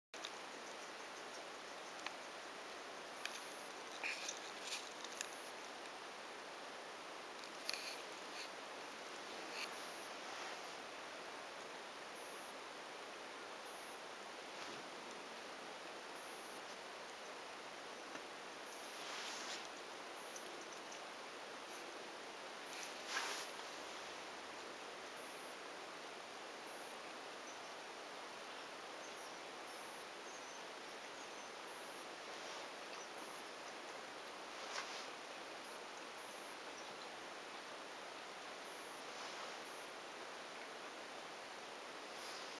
柴犬の赤ちゃん生後53日目
首輪をつける練習を始めました。とても鬱陶しそうで、嫌がります。最初から着けっぱなしにするのでなく、1～2時間程度着けたら外してやります。